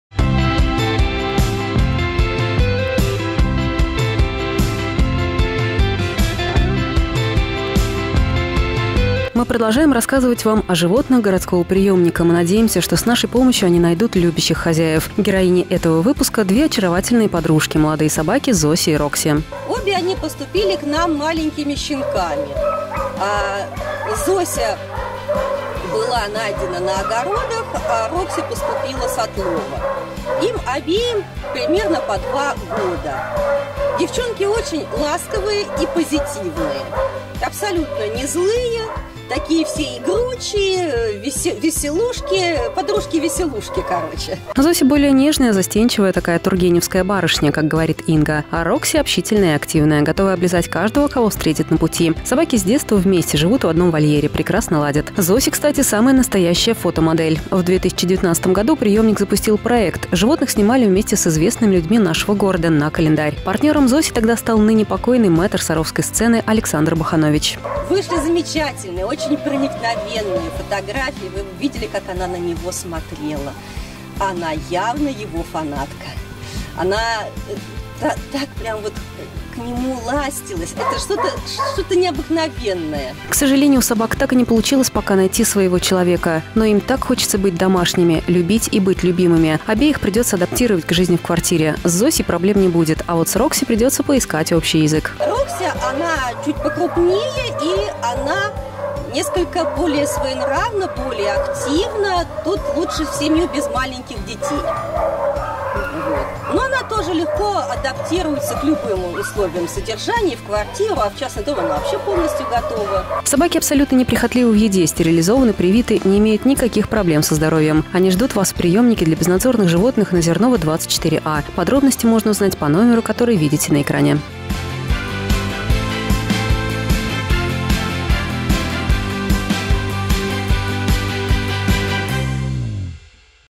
Ищу хозяина. Выпуск №6
Мы продолжаем рассказывать о животных городского приёмника. Героями нового выпуска проекта «Ищу хозяина» станут собаки Зося и Рокси.